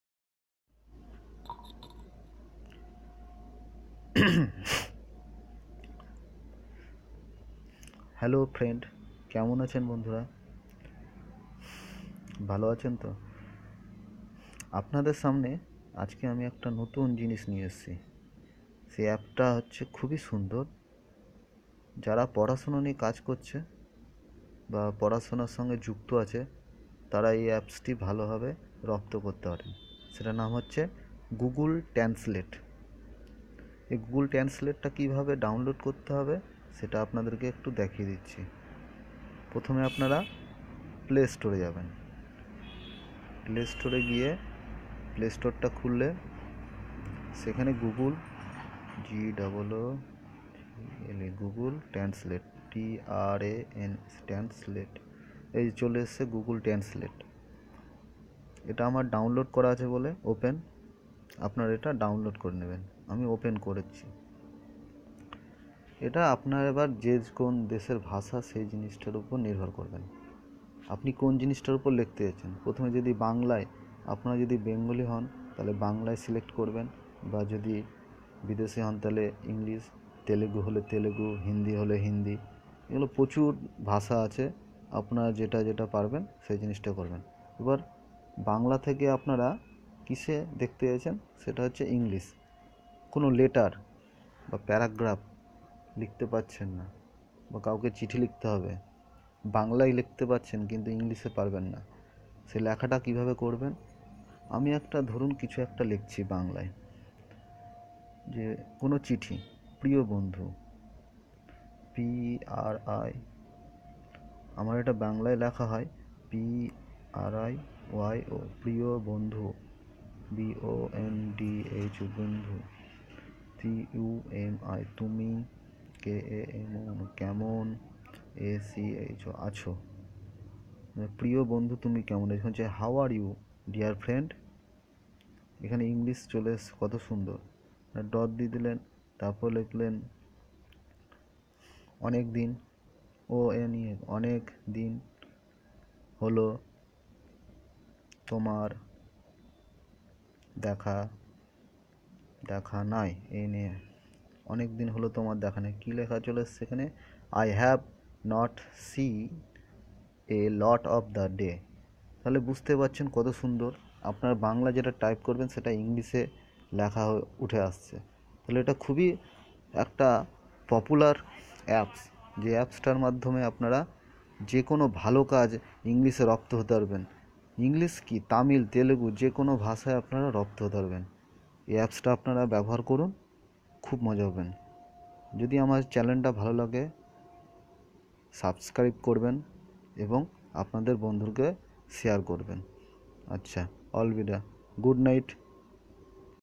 ইংরেজি শেখার সহজ পদ্ধতি 100%
এই আপ এর মাধমে আপনারা সহজে ইংরেজি শিখতে পারবেন। শুধু ইংরেজি নয় তামিল,তেলেগু,পাঞ্জাবি,হিন্দি আরো প্রচুর ভাষা শিখতে পারবেন।